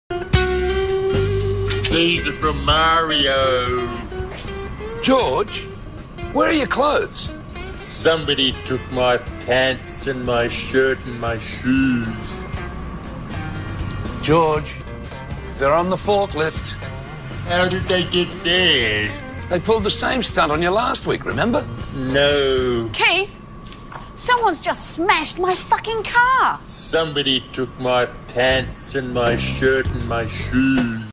Swift and Shift Couriers: Somebody took my pants and my shirt and my shoes
A funny clip of George, who had somebody took his pants, shirt and shoes.